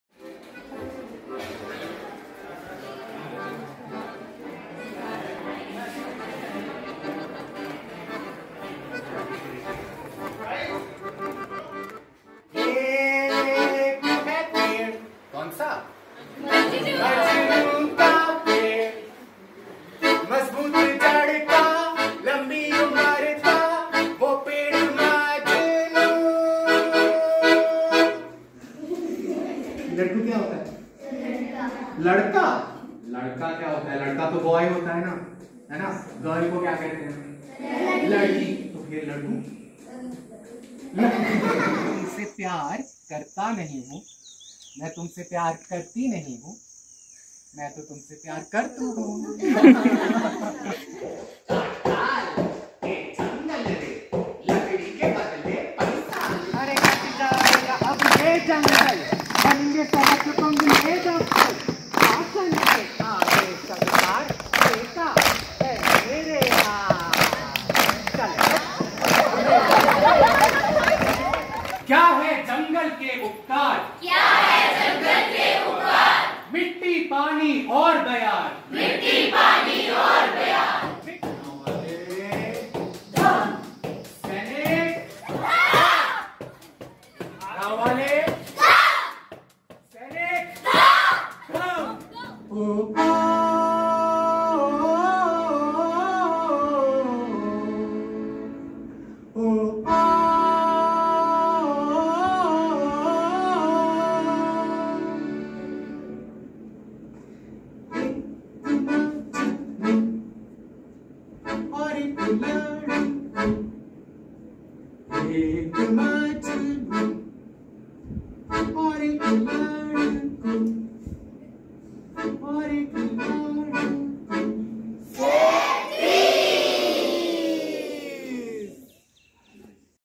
'Ek Majnu, Ek Ladku'🌳🌏🌈🏳‍⚧✨A storytelling performance
'Ek Majnu, Ek Ladku'🌳🌏🌈🏳‍⚧✨A storytelling performance by a colourfully queer artist, with lots of original songs, and music played on a piano accordion! 🪗🎶Touching on themes of gender and identity, forests and environment, this is a show that is as topical as it is entertaining. The performance gently highlights the struggle of queer folks, while celebrating the joy and power of accepting one’s true self. It also speaks of deep environmental crises from a space of creativity, resilience and hope🌈🏳‍⚧🌳🌏✨Free Entry, Donations are welcome!Writer, Composer and Performer: Anant Dayal Performance language: HindiDuration: 45 mins (approx)Age: For 10+ yearsFor registrations and enquiries : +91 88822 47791Follow the journey of this show as it travels to schools, colleges and community spaces across Dharamshala and beyond!